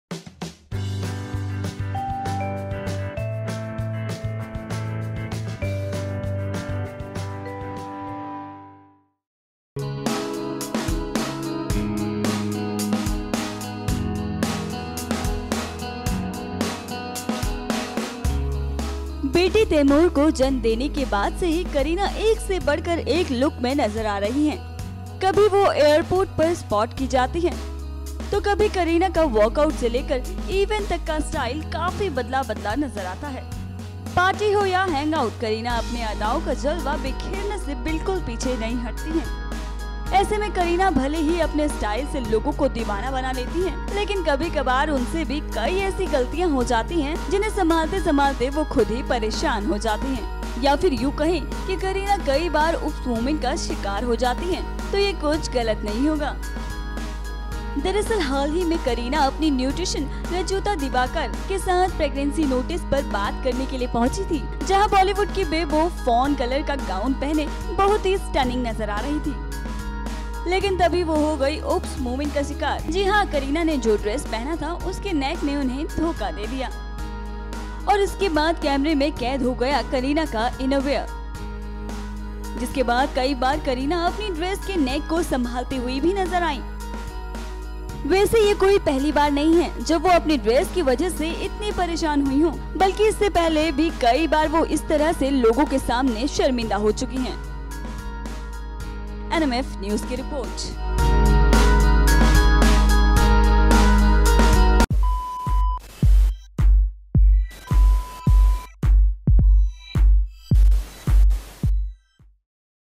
Kareena Kapoor हुई अपने कपड़ो से परेशान, देखिए जरा
kareena kapoor khan is look stunning day by day. kareena recently meet her nutrition planner  Rujuta diwekar.  on that time she wear a  forn color gown.